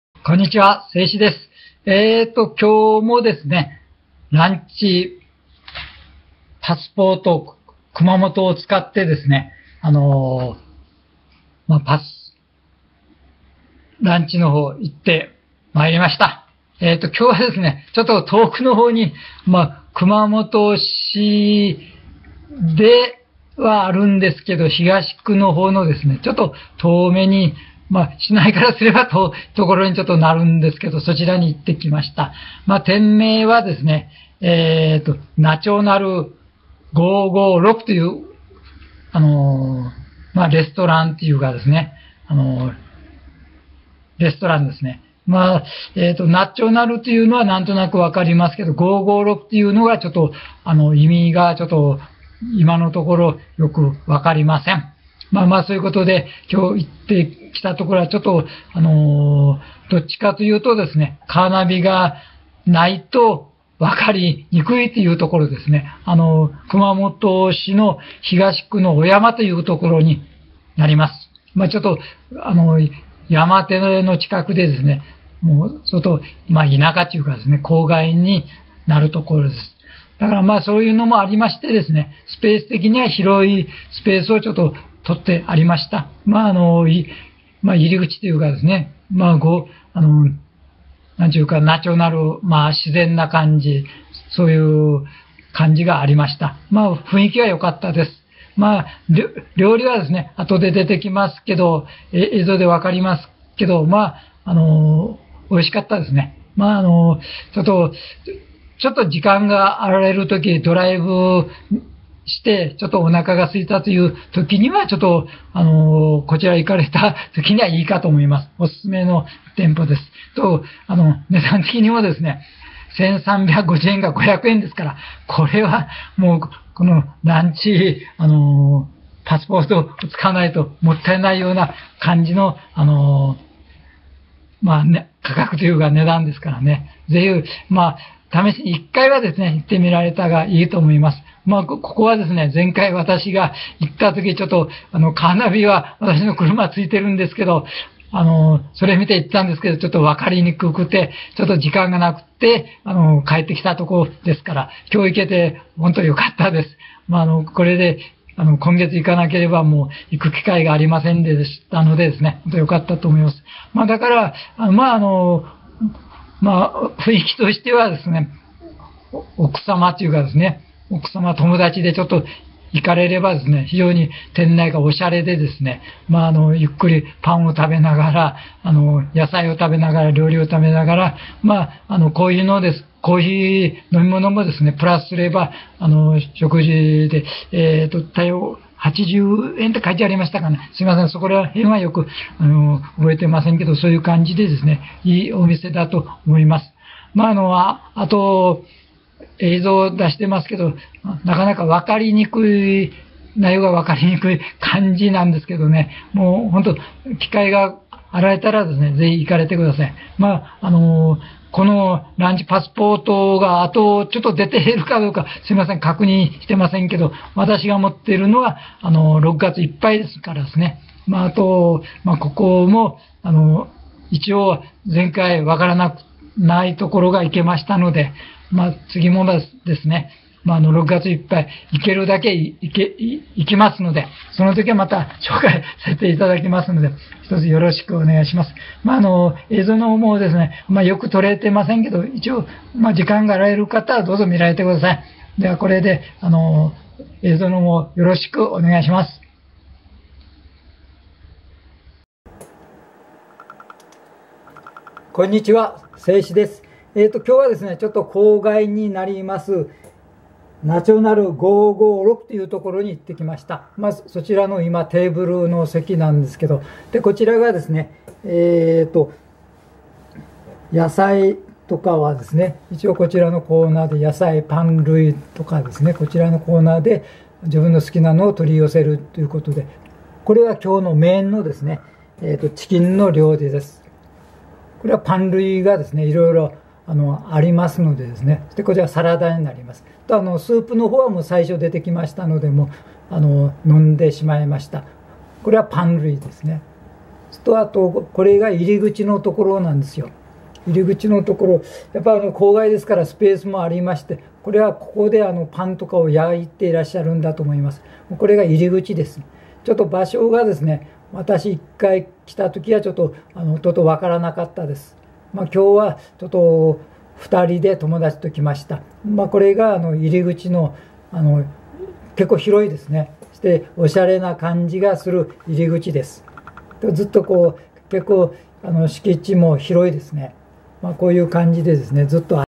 ランチパスポート熊本(^^♪natural５５６♪1,350円が500円でお買い得(^^♪です。
今回は、natural５５６♪もちろん、1,350円が500円です。
此処は、556週替りランチがパスポート熊本メニューです。
ナチュラナルゴーゴーロクは、中心部より離れています。
場所が分かり難いのでカーナビで探して下さいね(^^♪
お洒落な感じの作りの店内です、勿論外も素晴らしいでよ♫
ホントに得ですね(^^♪嬉しいですね(^^♪
皆さんも是非まだの方は、体験されて下さいね。
さあ~明日は、何処に行こうかな！？
撮影は、いつものSEICHIでした。